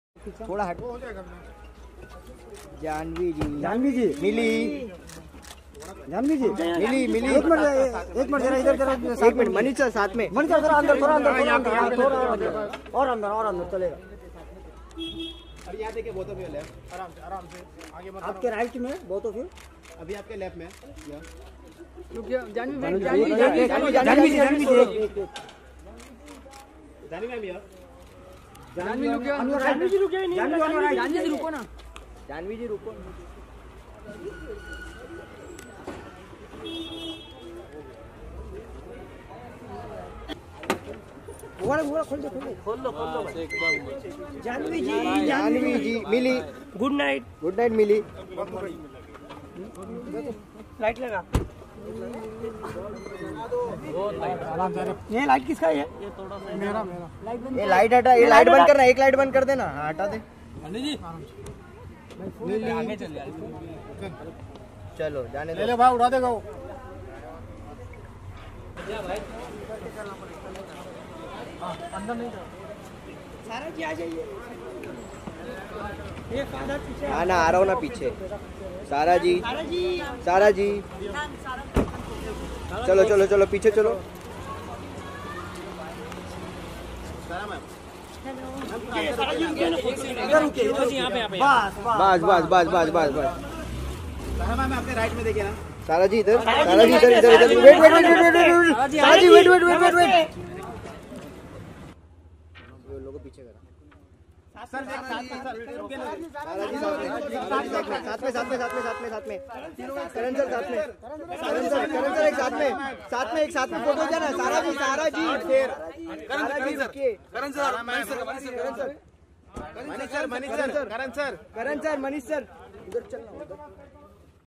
Janhvi Kapoor पोहची वरुण धवन के घर for Anniversary Celebration 🎉😍📸| Bollywood Social
'' Bollywood Social ''

Because in this Channel I Daily Upload Bollywood Interesting News Related.